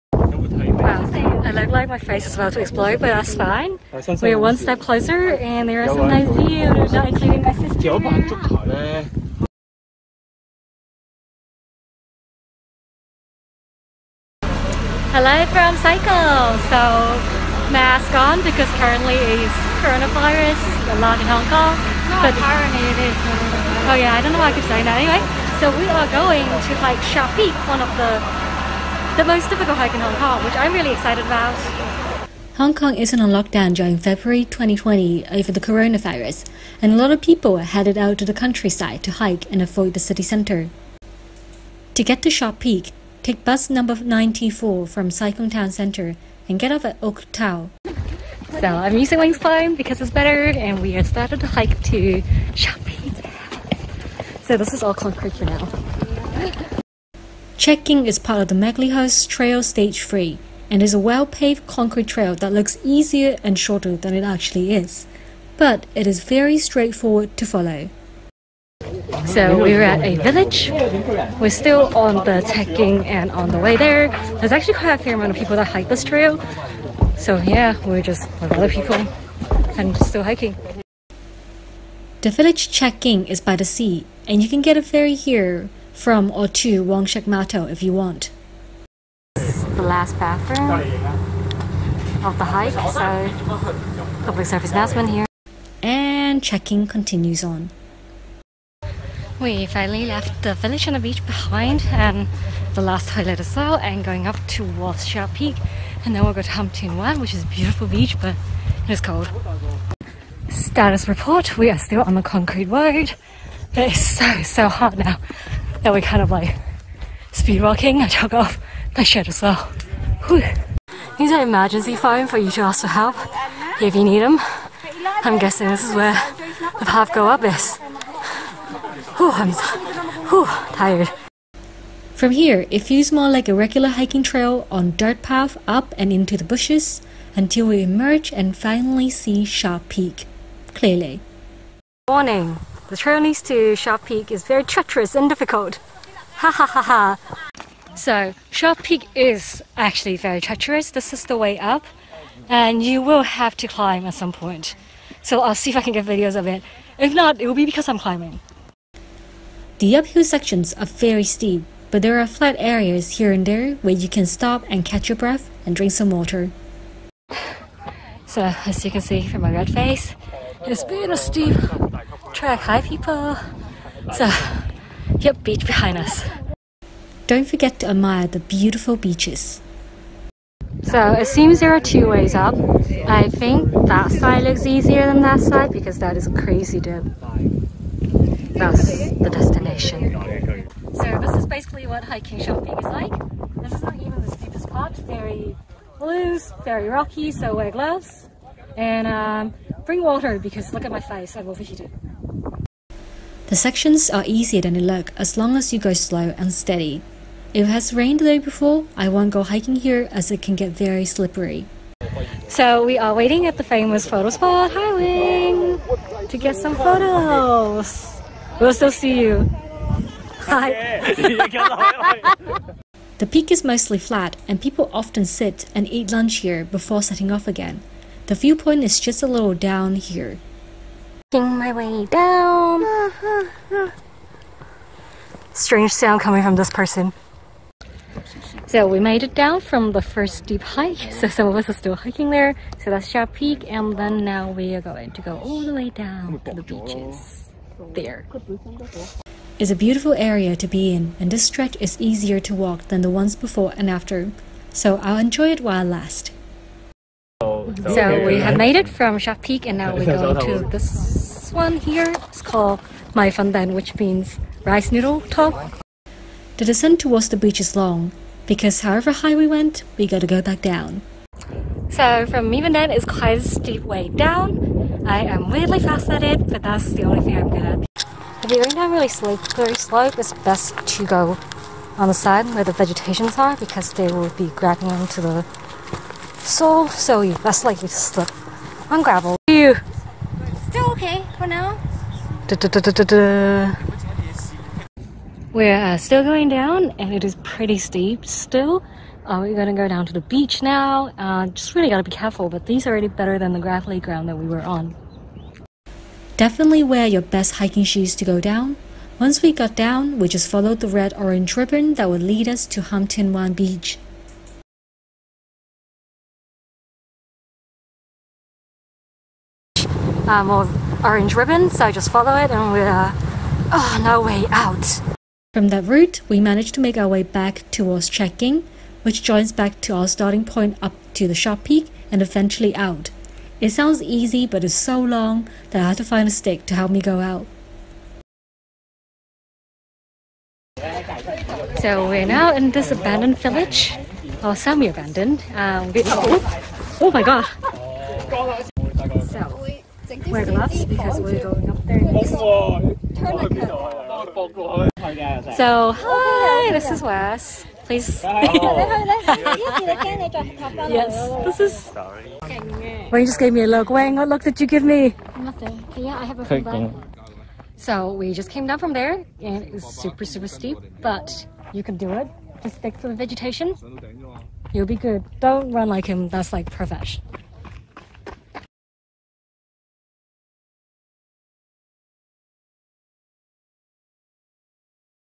Conquering Sharp Peak: the sharpest peak in Hong Kong
Sharp Peak is one of the hardest trail to conquer in Hong Kong. Located in Sai Kung, the trail takes 6-7 hours to finish and go pass some of the most beautiful beaches in Hong Kong.

It's absolutely essential to prep for the hike and wear good hiking boots!